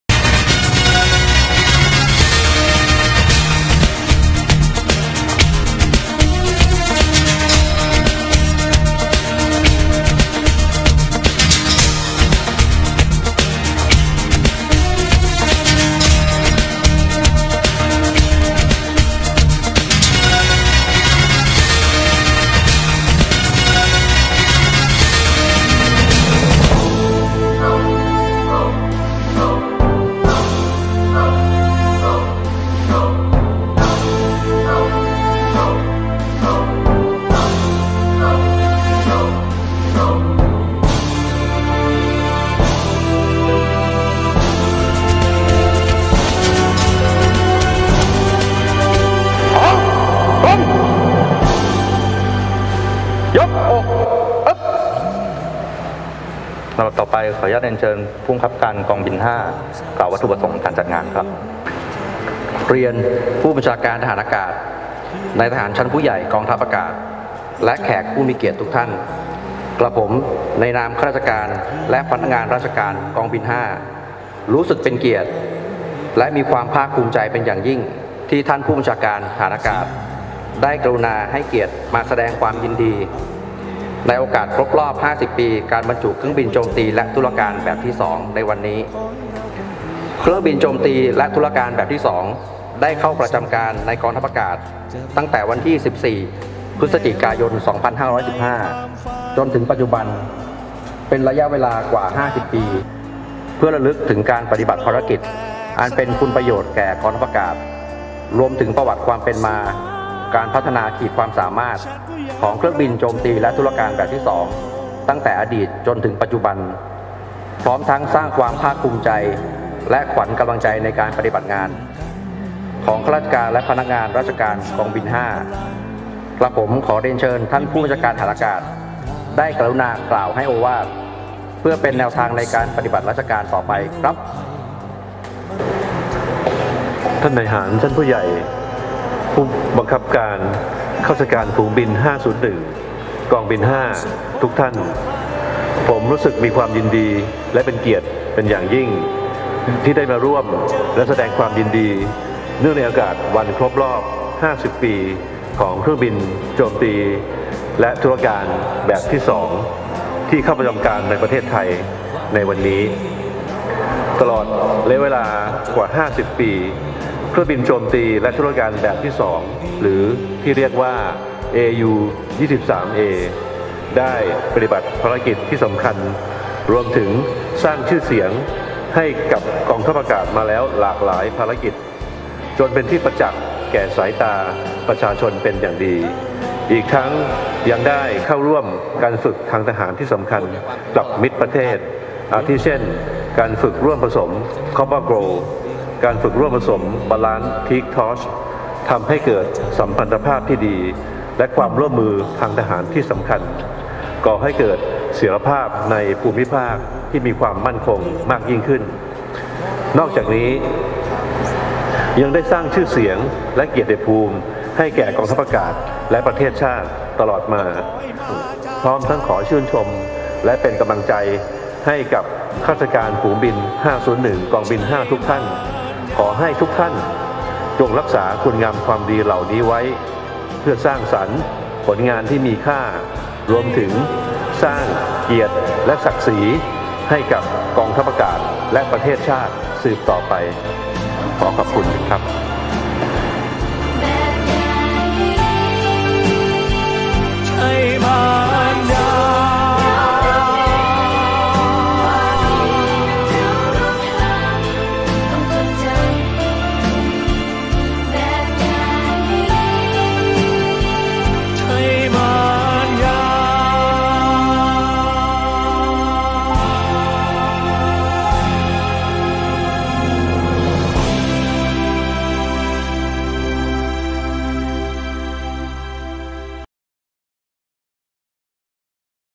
ผบ.ทอ.ประธานพิธีครบรอบ 50 ปี ในการบรรจุเครื่องบินโจมตี AU-23A
พลอากาศเอก อลงกรณ์วัณณรถ ผู้บัญชาการทหารอากาศ เป็นประธานในกิจกรรมเนื่องในโอกาสครบรอบ 50 ปี ในการบรรจุเครื่องบินโจมตีและธุรการแบบที่ 2 (บ.จธ.2)หรือ AU-23A เข้าประจำการในกองทัพอากาศ พร้อมการดำเนินโครงการพัฒนาขีดความสามารถเพื่อให้มีความพร้อมในการปฏิบัติภารกิจที่ทันสมัย และสามารถยืดอายุการใช้งานของอากาศยานได้ต่อไป ณ ฝูงบิน 501 กองบิน 5 จังหวัดประจวบคีรีขันธ์